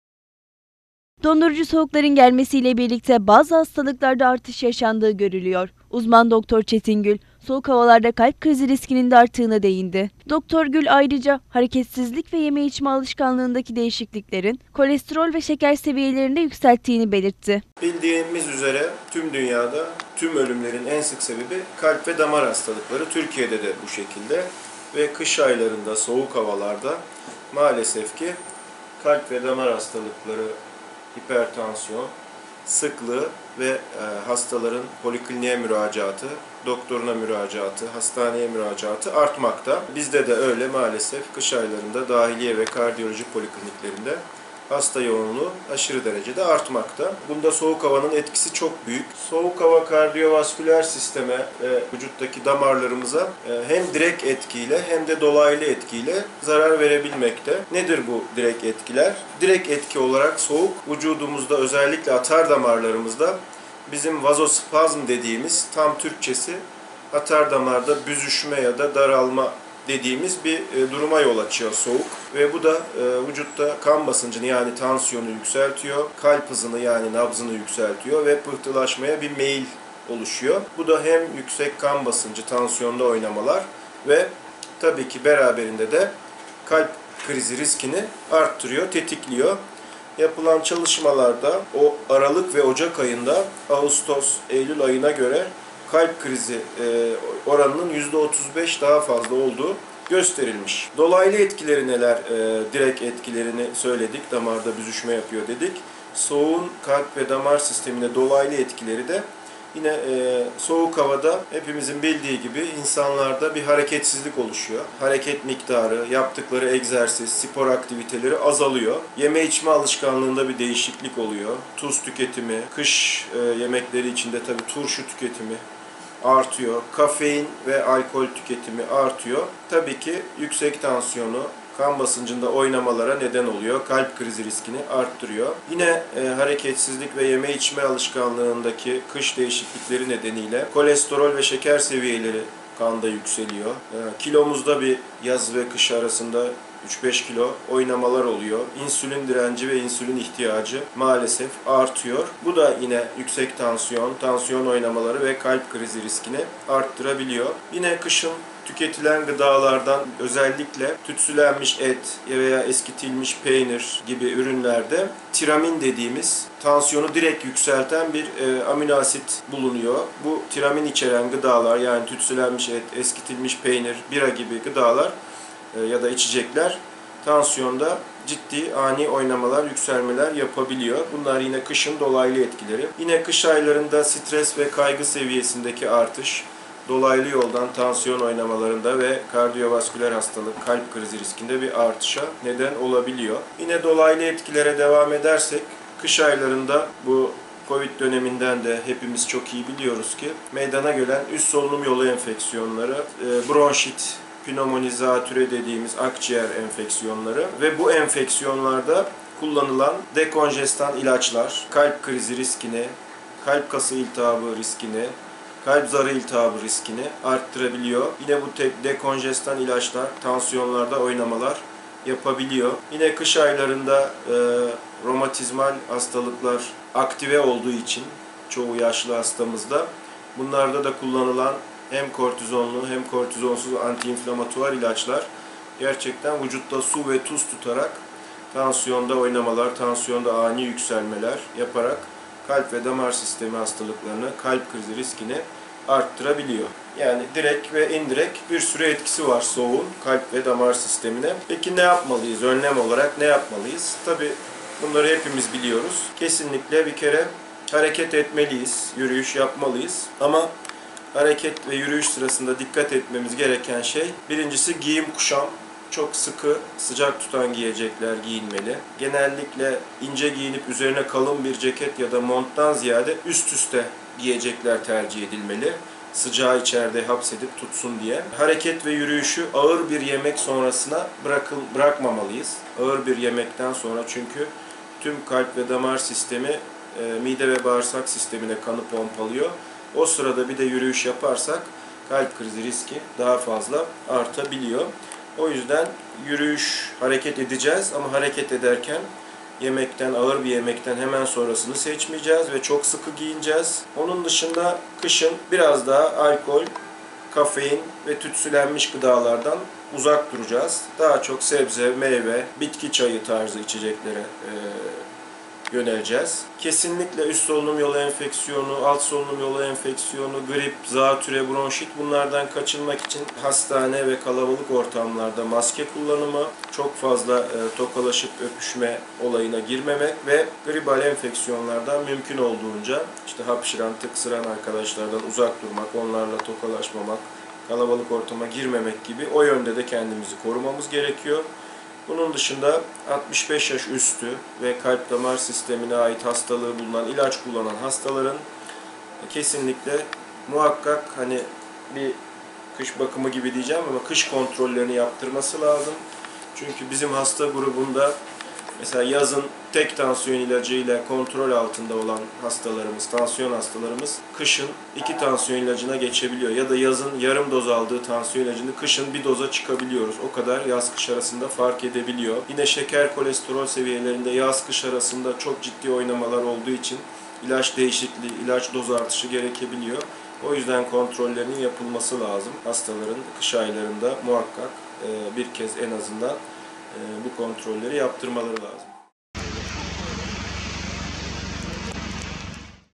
UZMANINDAN UYARI SOĞUK HAVALARDA KALP KRİZİ RİSKİ ARTIYOR
Kardiyoloji Uzmanı Dr. Çetin Gül, soğuk havalarda vatandaşların, kalp ve damar hastalıkları ve hipertansiyon şikayetiyle polikliniklere başvurularının arttığını açıkladı. Uzm. Dr. Gül, hipertansiyon ve kalp damar hastalıklarına soğuğun etkisinden bahsetti.